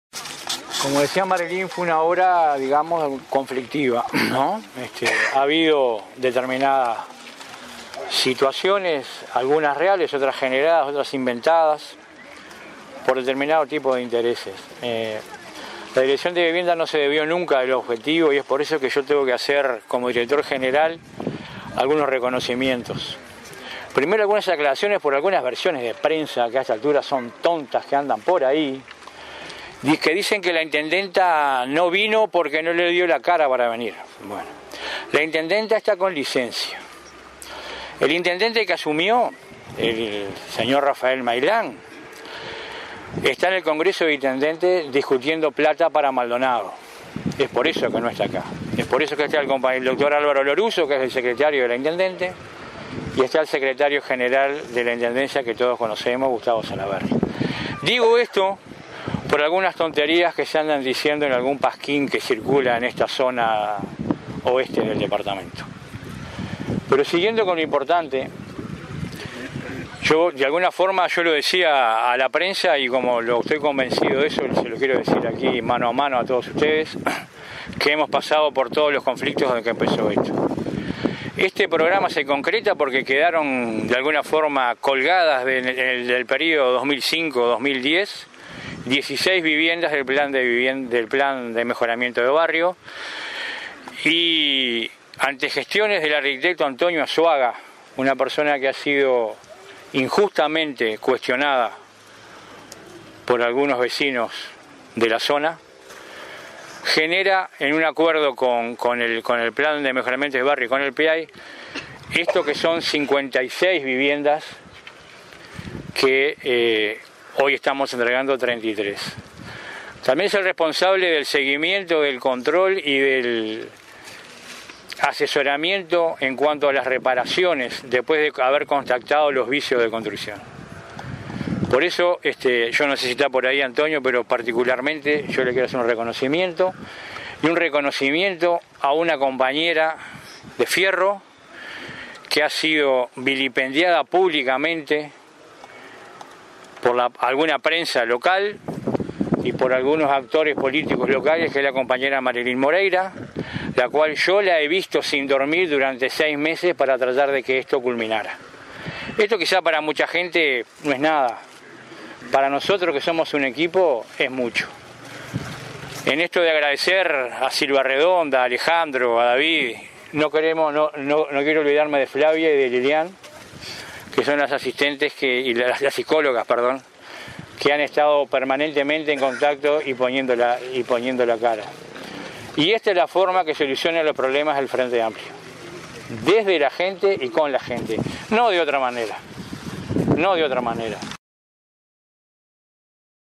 Director General Fernando Franco
El director de la oficina de Integración y Desarrollo Social de la Intendencia de Maldonado, Fernando Franco.